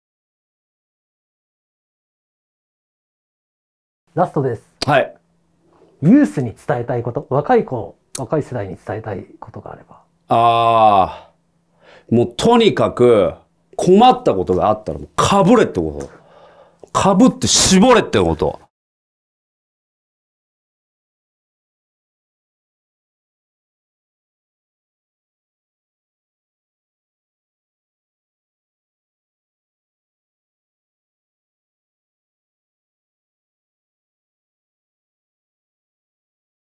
Mr.Parka jr. : ユースに伝えたいこと
Mr.パーカーjr.は、97年にデビュー。直後充電期間に入る。01年2度目のデビューも果たすが自らの方向性の違いを見つめ直す為に再び充電期間に入る。その後7度の充電期間を経て2020年実質デビュー。

Interviewer Bundai Yamada

■ニートtokyo■